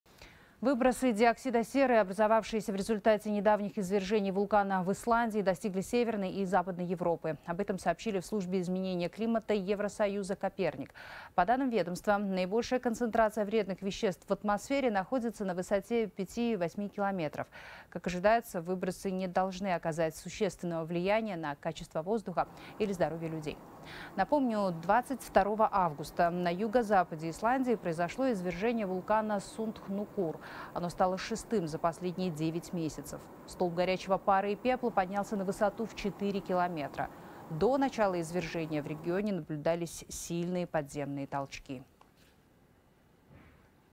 Токсичные выбросы из вулкана в Исландии накрыли Европу
Выбросы диоксида серы, образовавшиеся в результате недавних извержений вулкана в Исландии, достигли Северной и Западной Европы. Об этом сообщили в Службе изменения климата Евросоюза «Коперник».